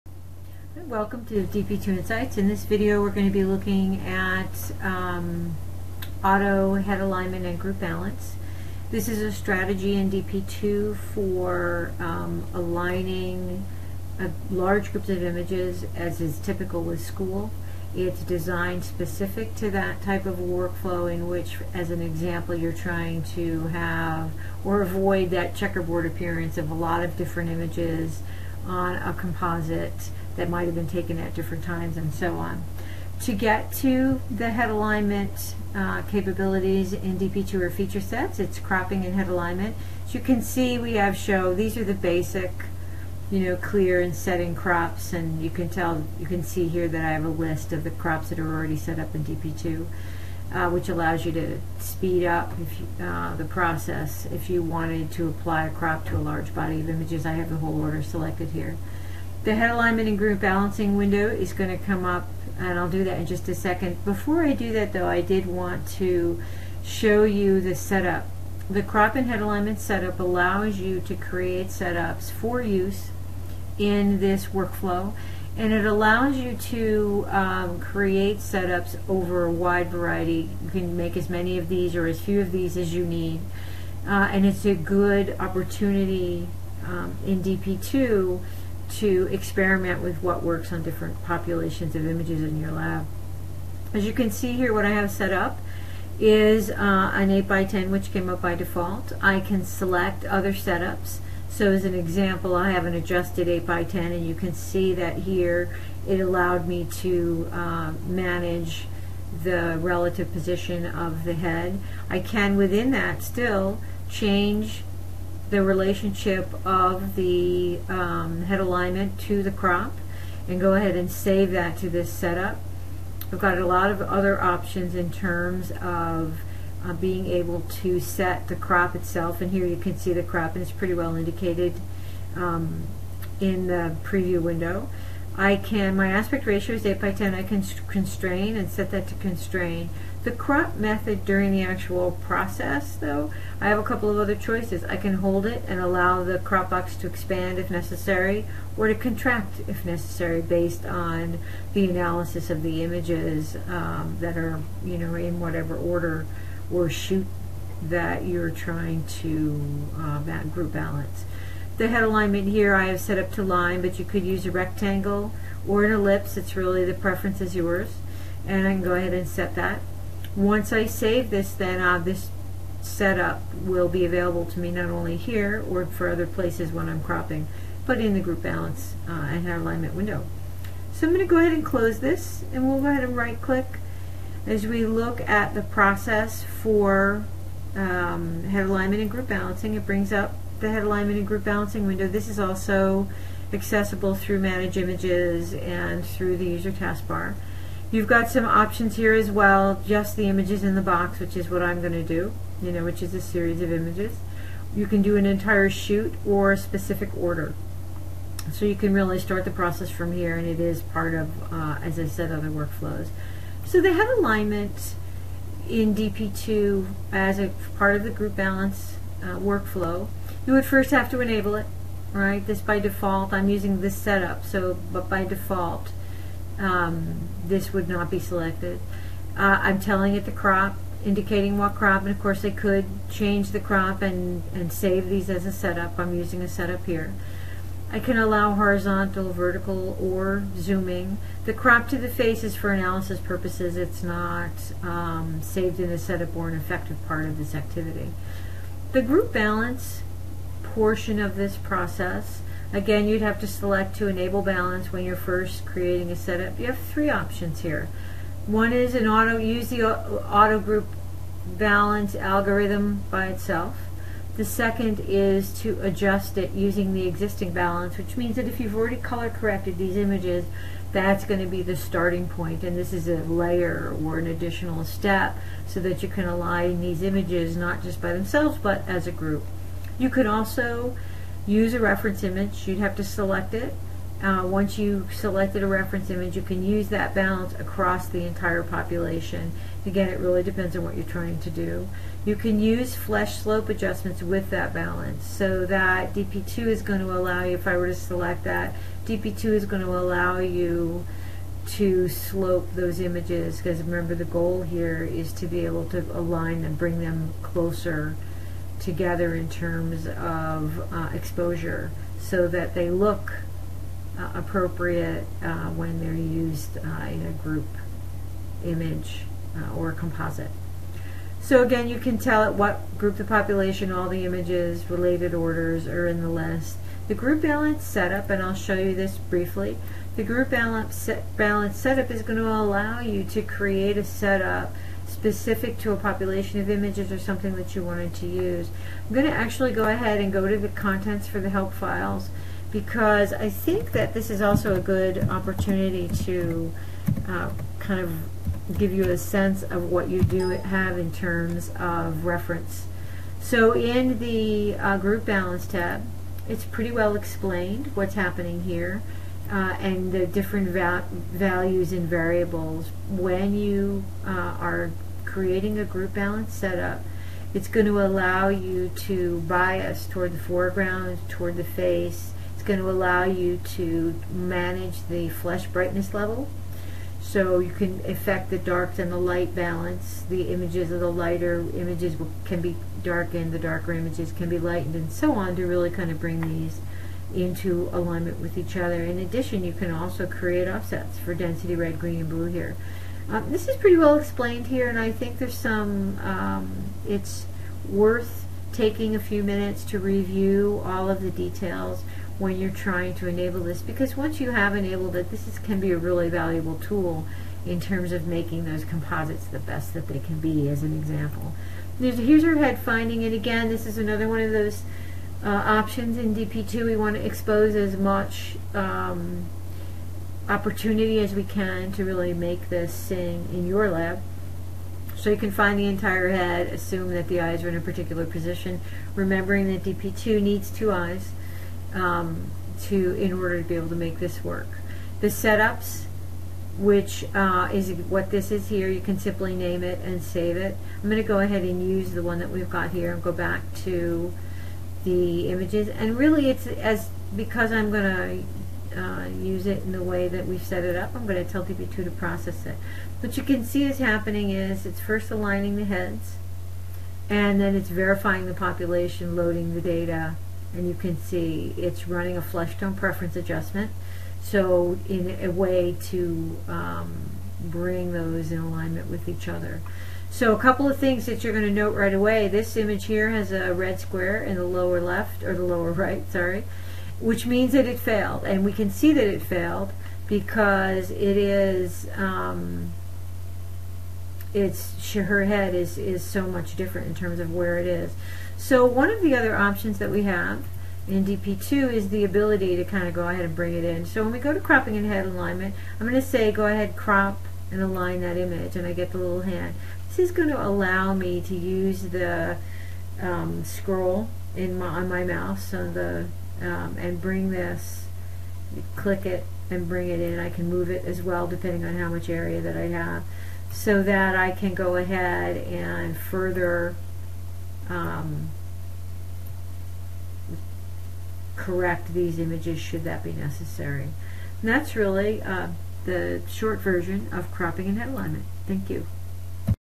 DP2 Head Alignment and Group Balance Workflow
Designed to help manage large volume “school” images; this brief overview includes automated head alignment and group balance features – both foreground and background. Images used in packages, service items, composites or other template driven workflows.